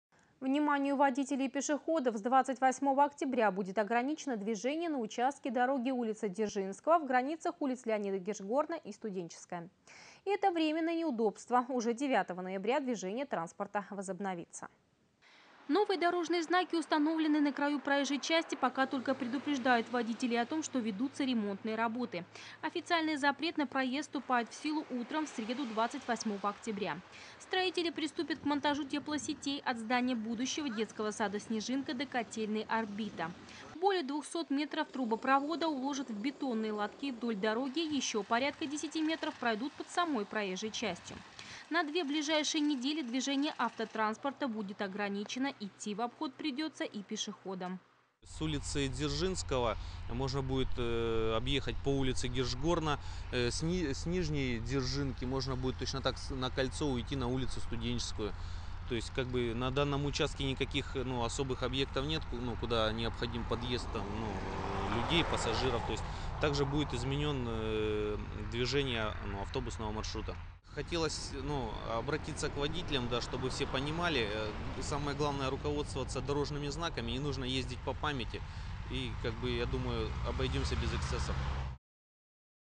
Вниманию водителей и пешеходов!
С 28 октября закрыто движение по улице Дзержинского от ул. Ленида Гиршгорна до ул. Студенческой.